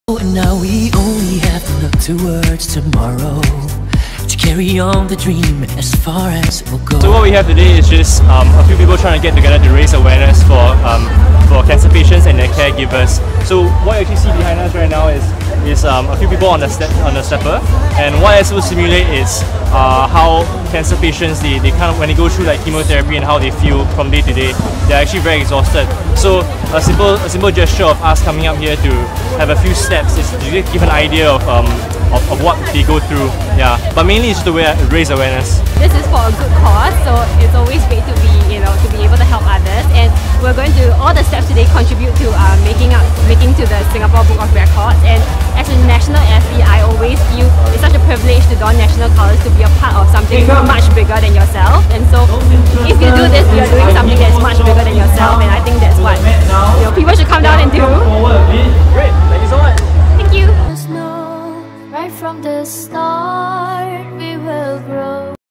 GetActiveSG 2019: Athletes with Voices - Raising Awareness for Cancer
Team Singapore athletes Michelle Sng, Christopher Cheong and Sasha Christian came together to raise cancer awareness through pledging steps, as part of Team Singapore's initiative called "Athletes with Voices".

Using sport as a force for good, the athletes helped to raise awareness not just for patients, but also for Adolescents Young Adults and their caregivers.

A new Singapore national record for the most number of cumulative steps recorded by steppers in 8 hours was set, as athletes from athletics, football, netball, kickboxing, silat, squash, volleyball, canoeing, basketball, Jiu Jitsu, and shooting lent their support amongst many others.

The total number was 344,744 after 8 hours, a new Singapore record.

+ Production Crew +
Director & Producer - Ong Kah Jing
Editor - John Yeong
Cameras - Ong Jun Xiang